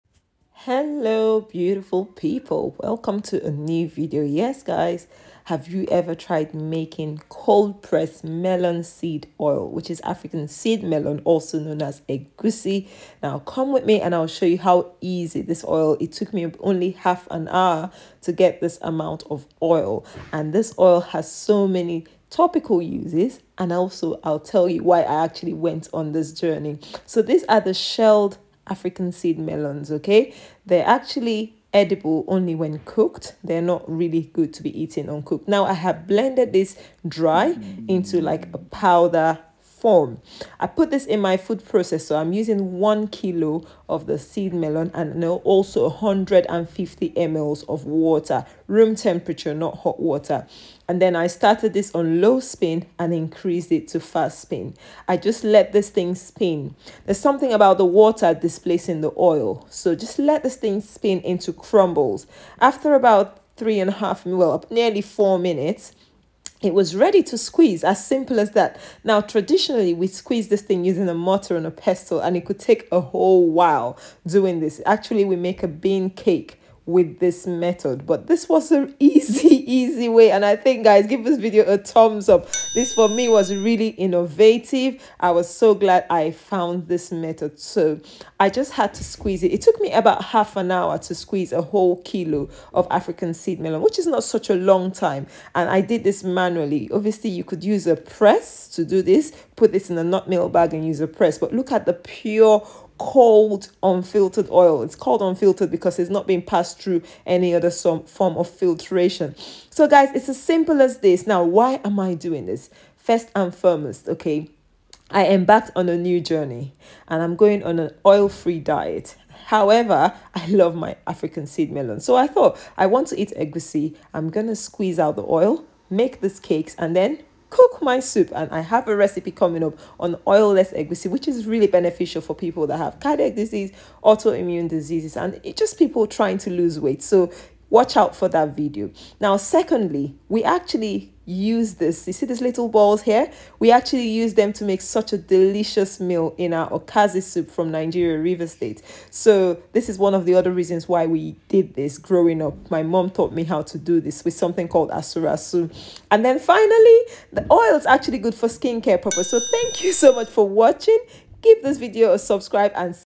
EASIEST WAY TO MAKE || COLD PRESSED SEED OIL EGUSI OIL AFRICAN SEED MELON
fatty acid profile of egusi melon oil. palmitic (13.5%), stearic, (13.7%), oleic (14.5%) and linoleic, (56.9%) are the main fatty acids present in egusi melon.